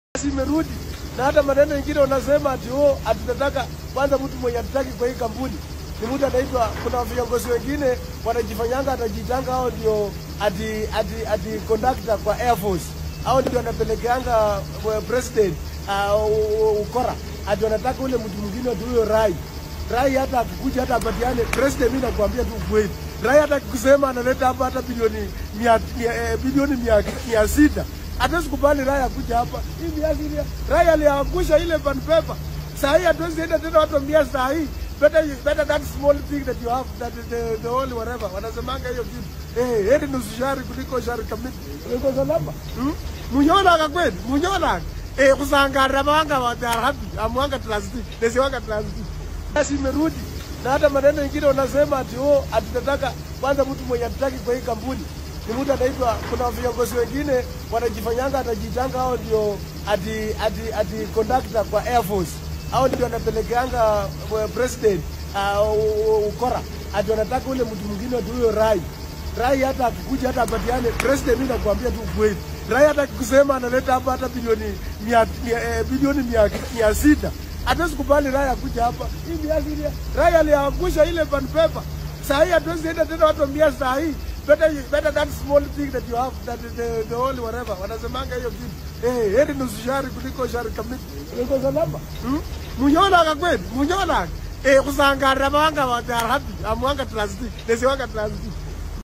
WEWE RUTO SISI HATUTAKUBALI RAI HAPA MUMIAS,ALIANGUSHA PANPAPER salasya says
"Wewe president acha nikuambie ukweli,hao watu wanajifanya makanga kwa ndege wanakudanganya,sisi hatuwezi kubali huyo Rai mwenye aliangusha Pan paper" Mp Peter Salasia tells  President Ruto over changing Mumias Sugar Company Investor